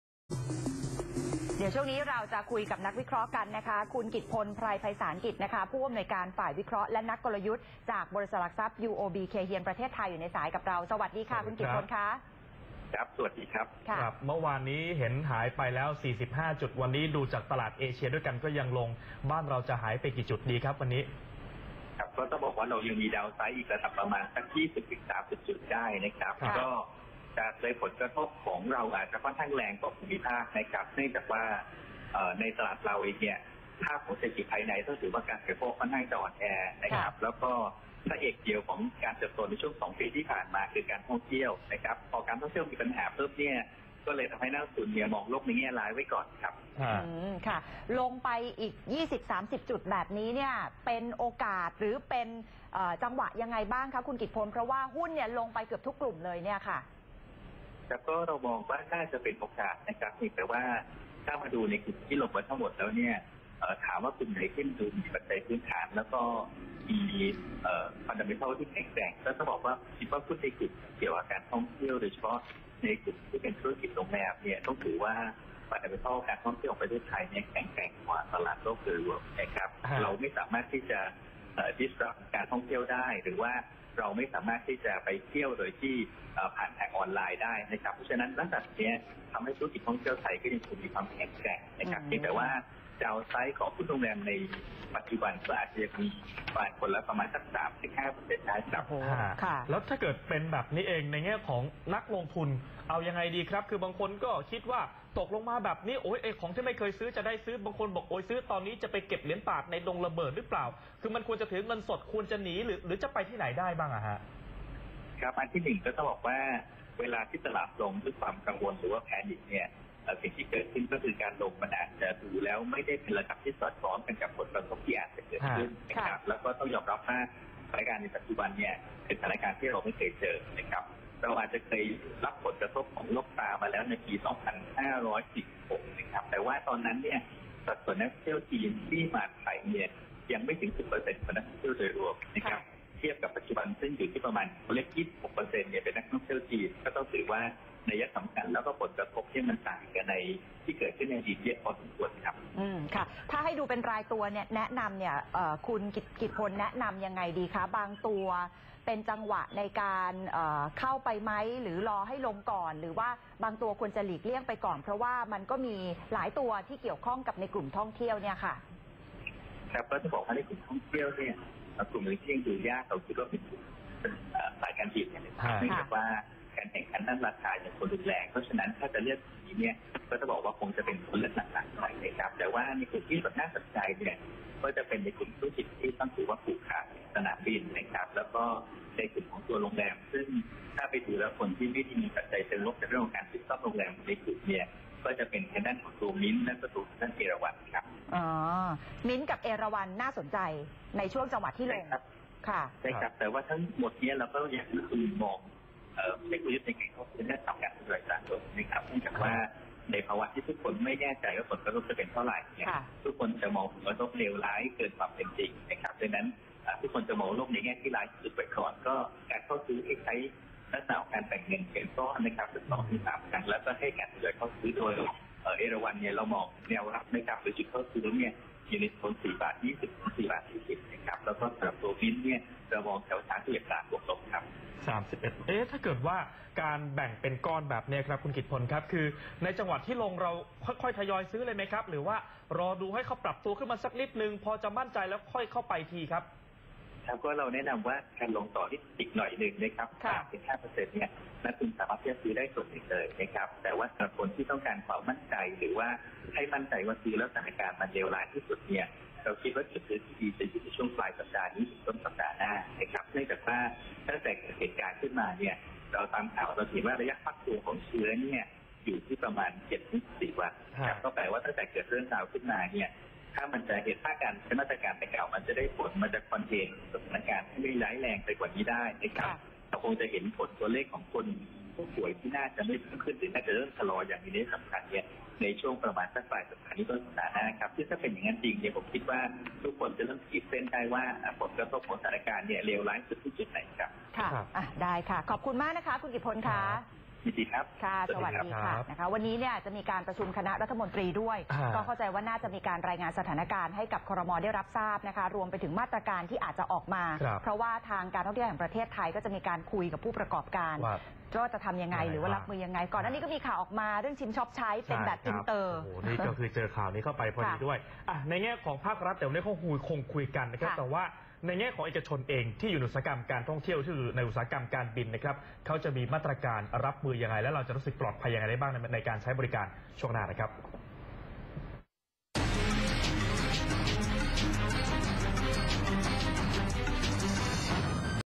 "โคโรนา" กระทบหุ้นไทย Iชั่วโมงทำเงิน I 28-01-63
"โคโรนา" กระทบหุ้นไทย I ชั่วโมงทำเงิน I 28-01-63
วิเคราะห์หุ้นไทยร่วงระนาววานนี้-เลือกลงทุนกลุ่มไหนดี
กับคุณกิจพณ ไพรไพศาลกิจ ผอ.ฝ่ายวิเคราะห์และนักกลยุทธ์
บล.ยูโอบี เคย์เฮียน(ประเทศไทย)